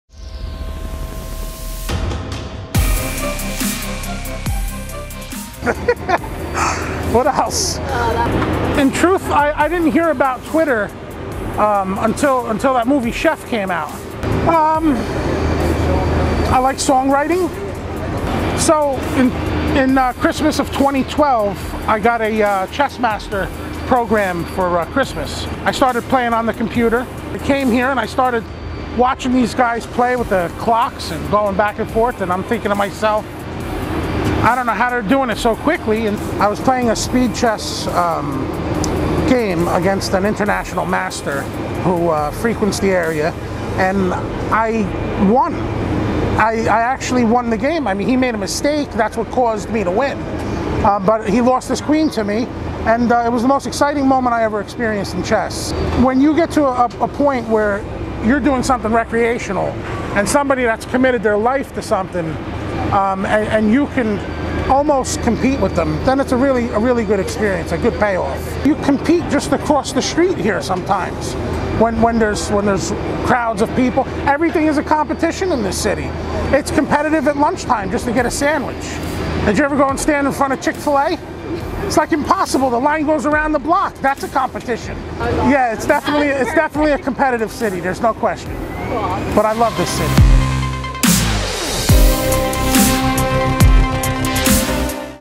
VPM Game Time: Episode 1, Check Mate
In episode 1, meet one of Bryant Park's finest chess players. Whether it’s on the boards, or fighting to get a seat at the park during lunchtime, or beating the line at Chik-fil-a, this guy understands just how insanely competitive New York City is at every hour of every day, and knows what it takes to keep up.

Summer 2016 Marketing Campaign – Compete Every Day